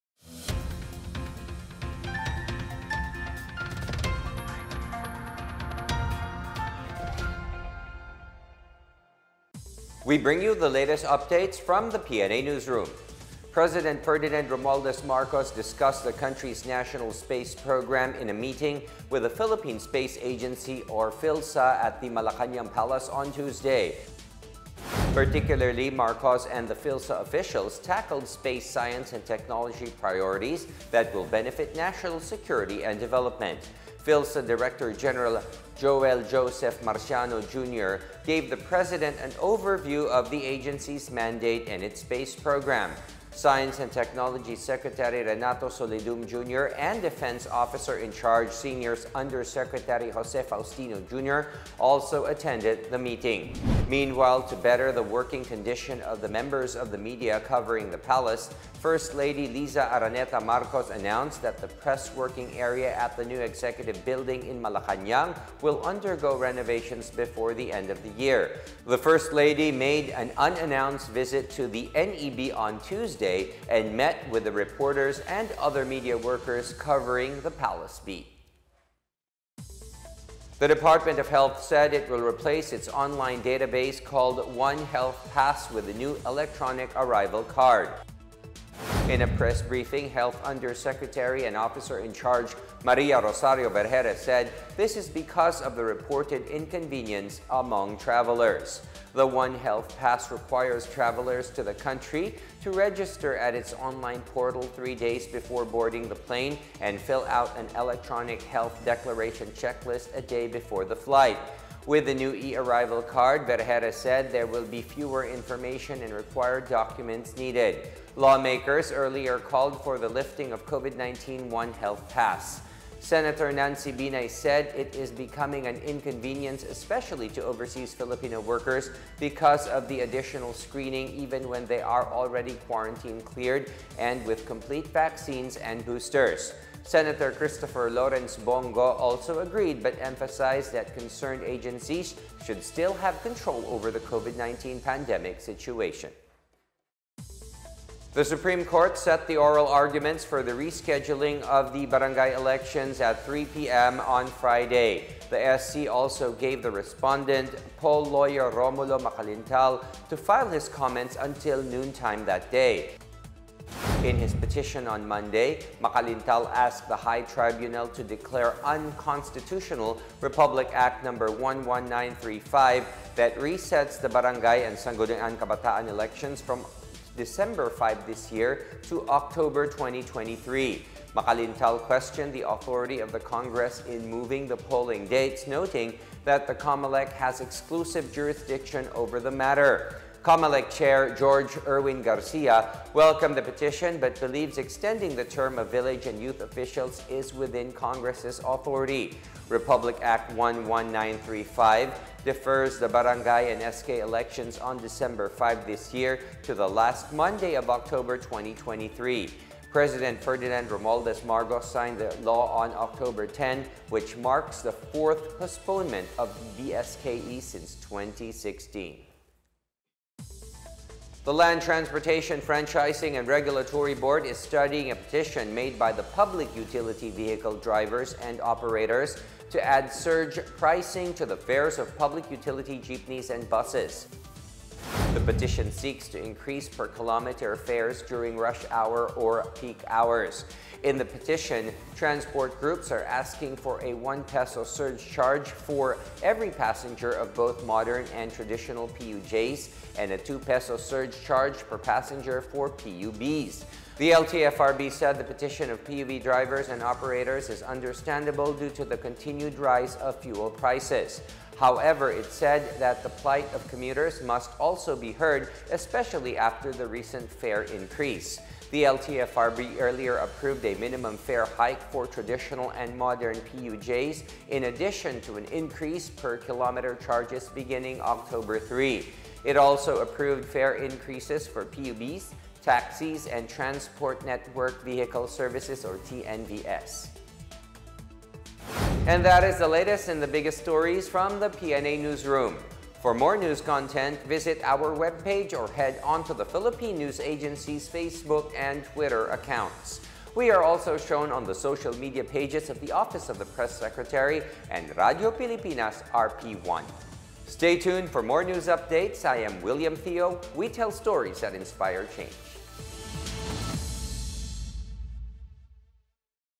PNA NEWSROOM UPDATES - 1 (2022.10.19)
WATCH: PNA NEWSROOM UPDATES

• Marcos, PhilSA tackle PH space program
• Palace press working area to undergo renovations: First Lady
• DOH replaces One Health Pass with eArrival Card
• Oral arguments on village poll reset start Oct. 21
• LTFRB studies petition for 'surge' charge on PUV fares